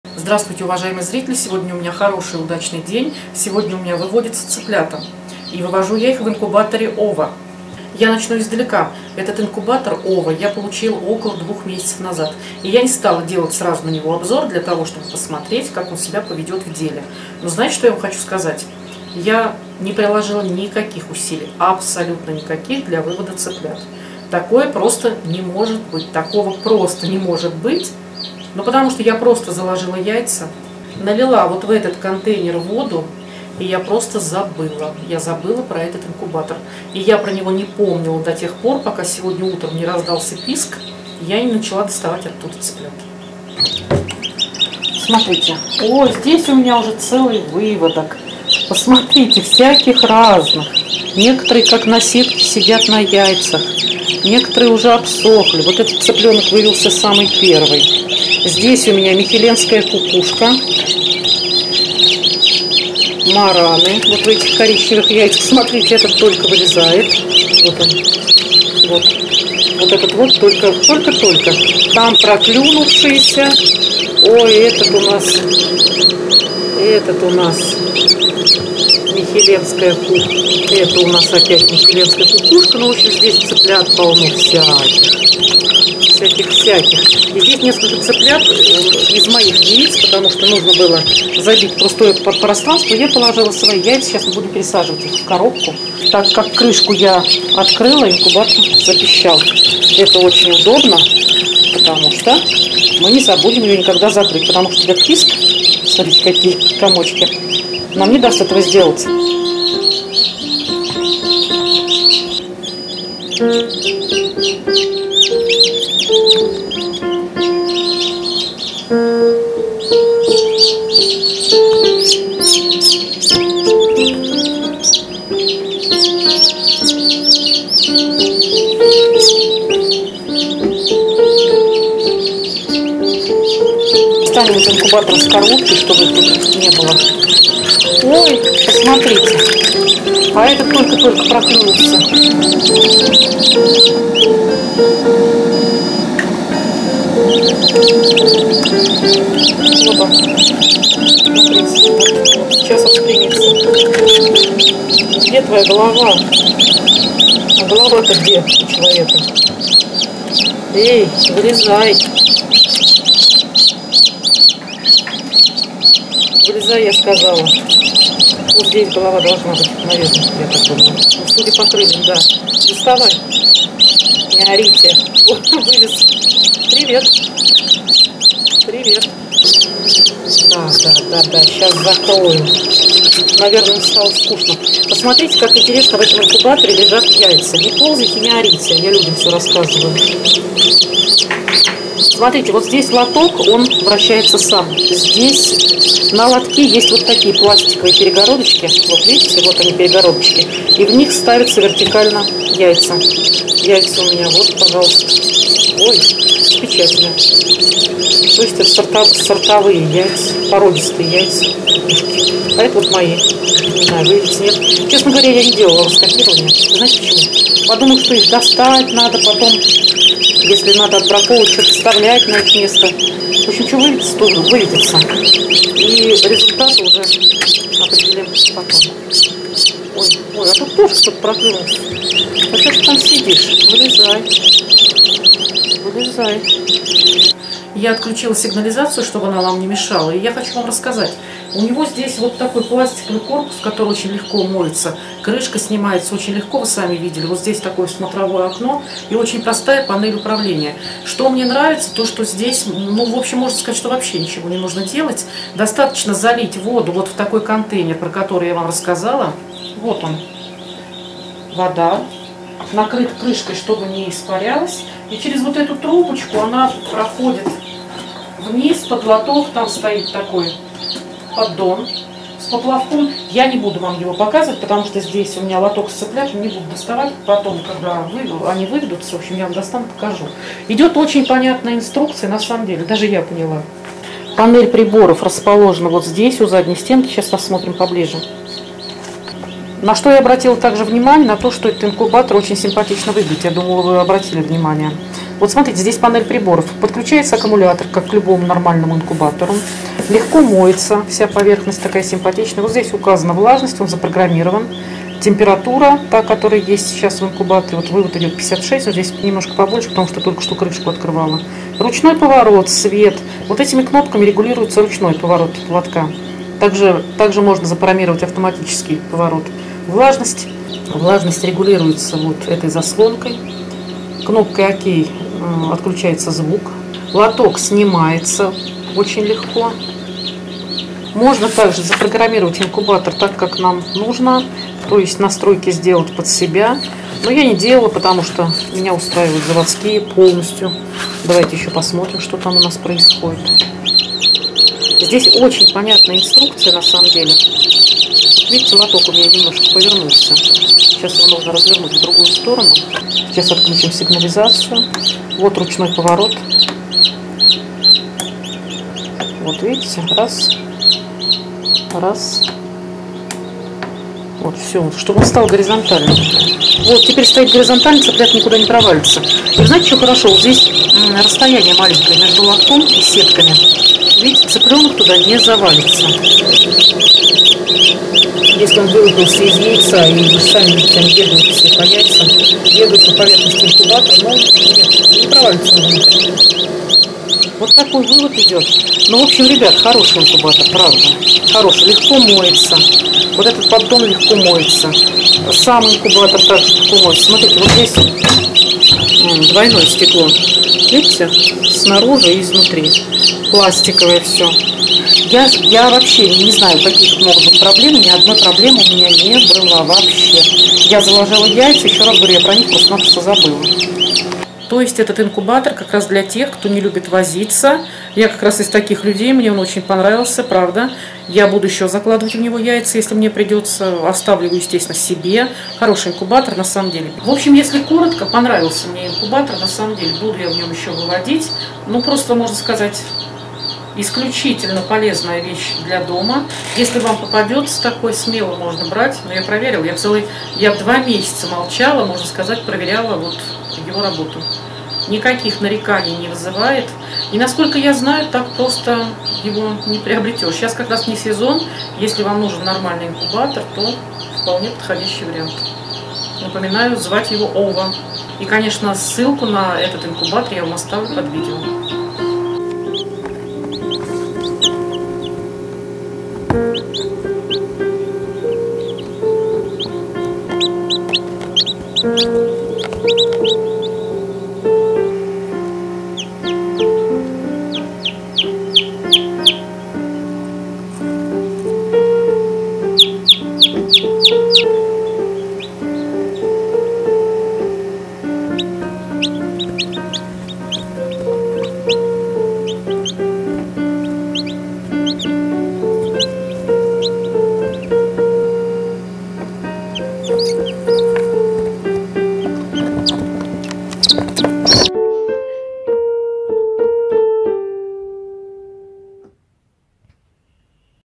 Инкубатор OVO Цыплята РАЗ ДВА и Готово
Я не увидела вообще никаких проблем, инкубатор выводит сам. я заложила куриные яйца, залила в контейнер воды , выставила настройки заводские. И просто ждала вывода